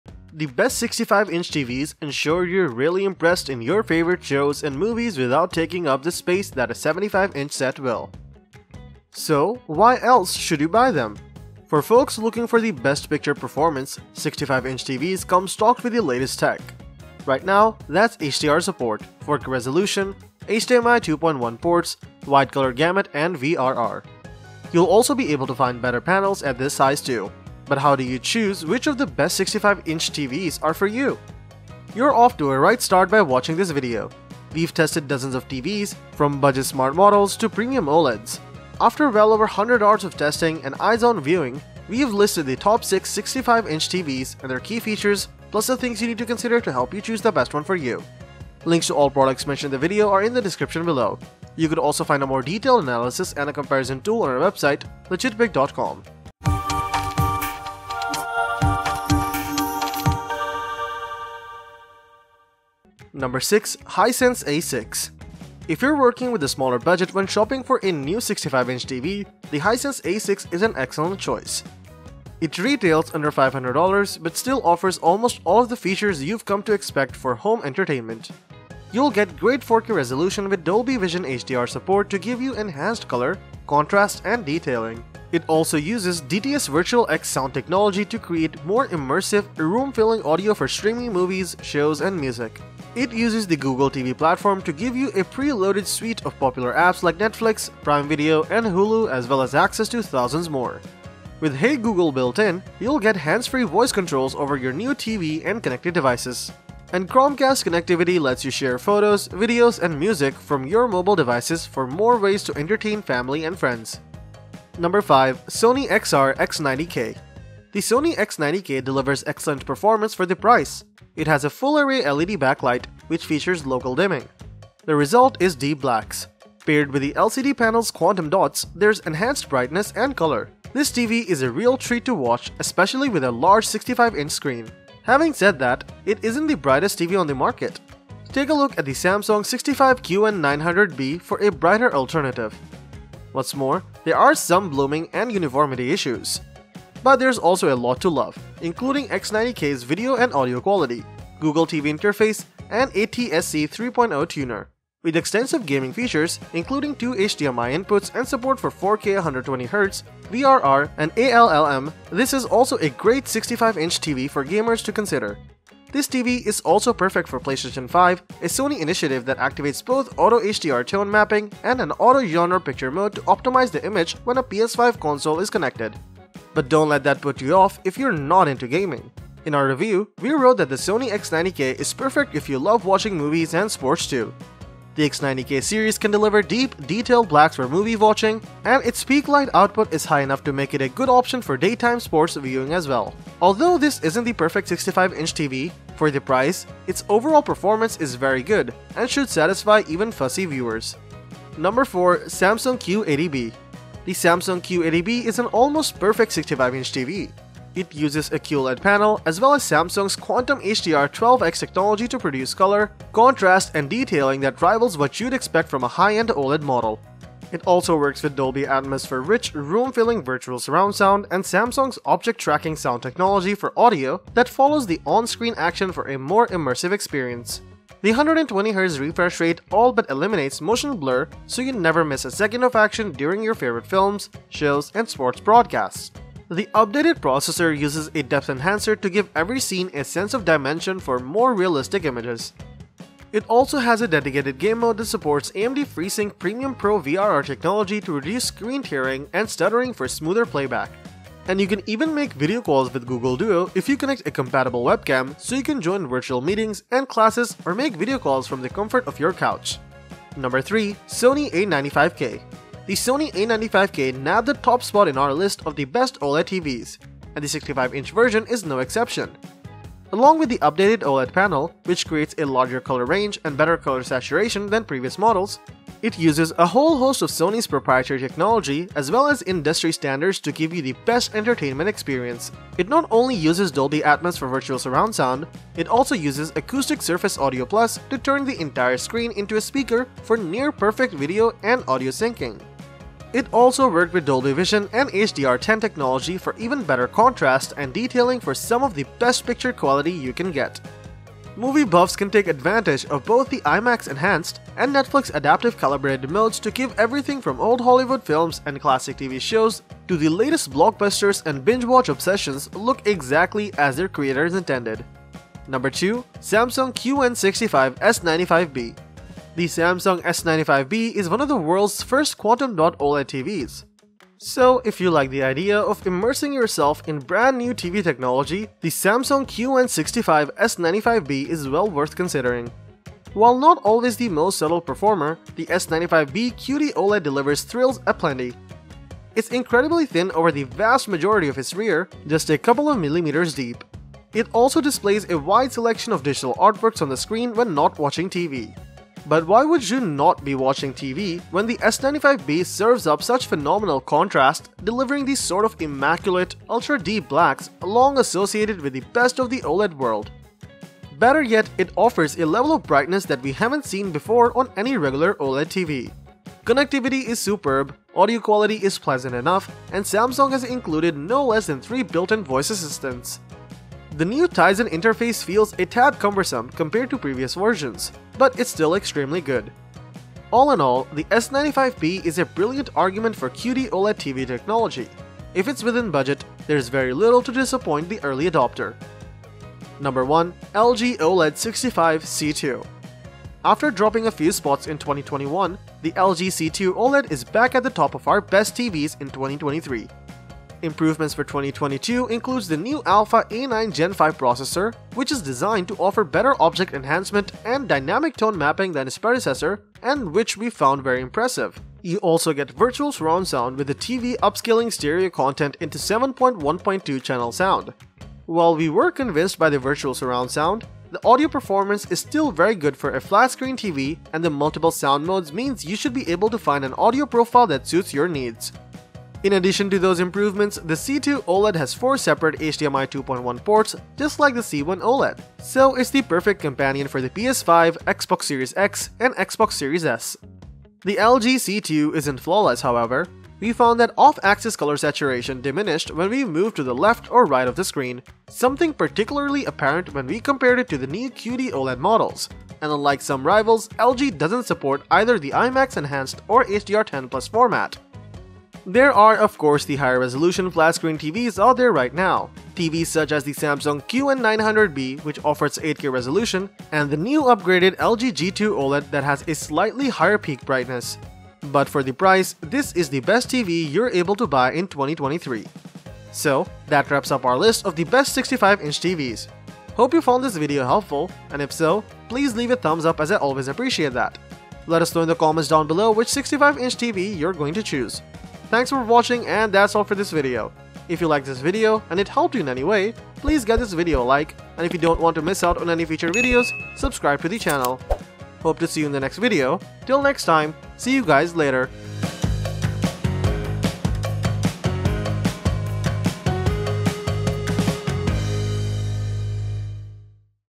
Best 65 Inch TV 2023 [don’t buy one before watching this]
► Links to the best 65 inch tv 2023 we listed in this video:

► 6. Hisense 65A6H (Amazon)
► 5. Sony XR X90K (Amazon)
► 4. Samsung Q80B (Amazon)
► 3. Sony XR65A95K (Amazon)
► 2. Samsung QN65S95B (Amazon)
► 1. LG OLED65C2 (Amazon)

Table of Contents:
0:00​​ - Introduction
1:04 - Hisense 65A6H
1:55 - Sony XR X90K
3:24 - Samsung Q80B
4:23 - Sony XR65A95K
5:16 - Sony XR A95K
6:30 - LG OLED65C2
7:56 - Conclusion

The best 65-inch TVs immerse you in your favorite shows and movies without taking up the space that a 75-inch set does.

So, why else should you purchase them? For those looking for the best picture quality, 65-inch TVs are equipped with cutting-edge technology. For the time being, that includes HDR support, 4K resolutions, HDMI 2.1 ports, Wide Color Gamut, and VRR. At this size, you'll also be able to find better panels (Mini-LED or OLED).